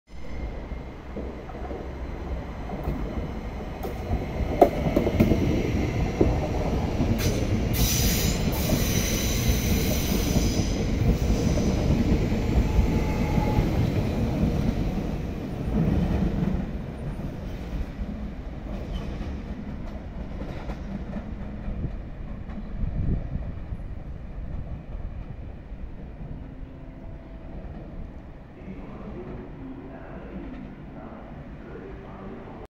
Ausfahrt BR 1429 aus Wuppertal Hbf
Wuppertal Hauptbahnhof mit Ausfahrt einer S-Bahn der DB-Baureihe 1429 ("Flirt 3"). Aufgenommen am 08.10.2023.